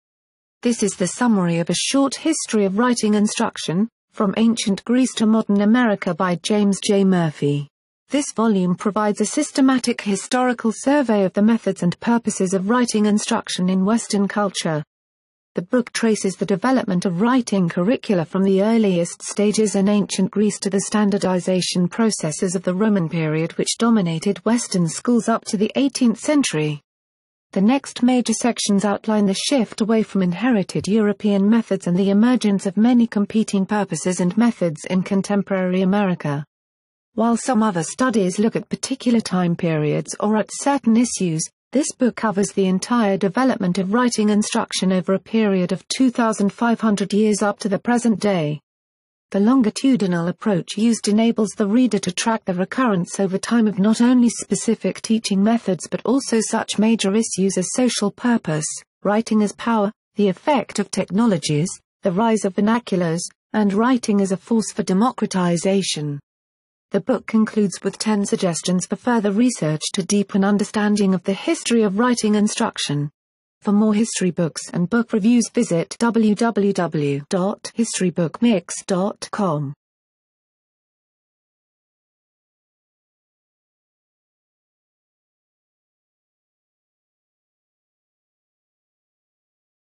History Book Review: A Short History of Writing Instruction: From Ancient Greece To Modern Americ...
This is the summary of A Short History of Writing Instruction: From Ancient Greece To Modern America by James J. Murphy.